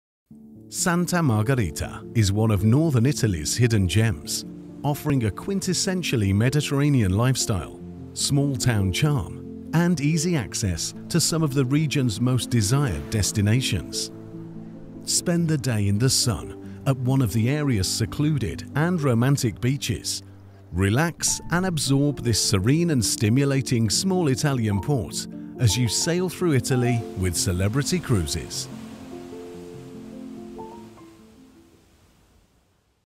Discover Santa Margherita With Celebrity Cruises
Santa Margherita is one of northern Italy’s hidden gems, offering a quintessentially Mediterranean lifestyle, small-town charm, and easy access to some of the region's most desired destinations.

Spend the day in the sun at one of the area’s secluded and romantic beaches. Relax and absorb this serene and stimulating small Italian port as you sail through Italy with Celebrity Cruises.